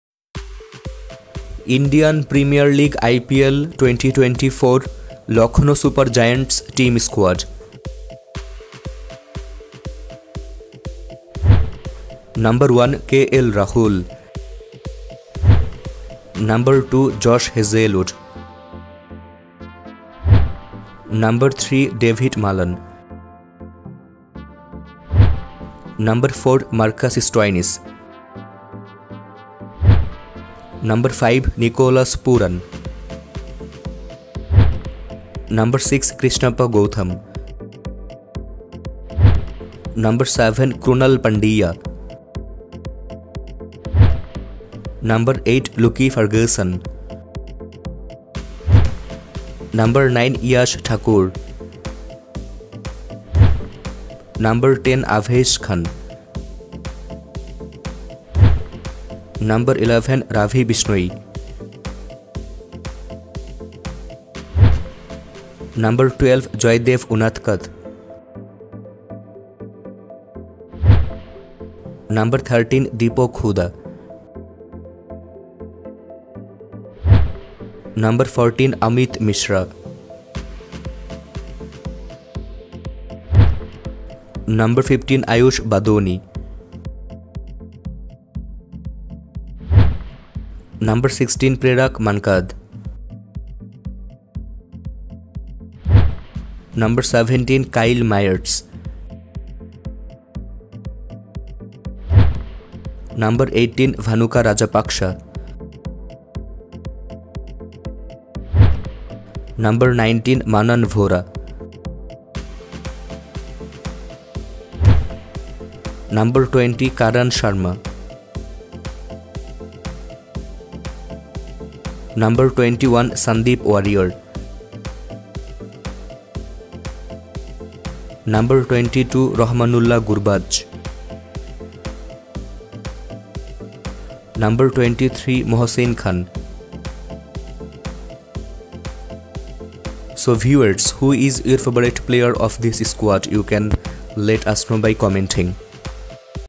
IPL 2024 - Lucknow Super Giants New Squad | Lucknow Team Players List 2024 | LSG Team Squad 2024
Lucknow Super Giants Team Squad

1. KL Rahul
2. Josh Hazelwood ✈️
3. Dawid Malan✈️
4. Marcus Stoinis✈️
5. Nicholas Pooran✈️
6. Krishnapa Gowtham
7. Krunal pandya
8. Lukie Ferguson ✈️
9. Yash Thakur
10. Avesh khan
11. Ravi Bishnoi
12. Jaydev Unadkat
13. Deepak Hooda
14. Amit Mishra
15. Ayush Badoni
16. Prerak Mankad
17. Kyle Mayers✈️
18. Bhanuka Rajapaksa✈️
19. Manan Vohra
20. Karun sharma
21. Sandeep Warrier
22. Rahmanullah Gurbaz ✈️
23. Mohsin Khan
24. Udhvir Singh

Thanks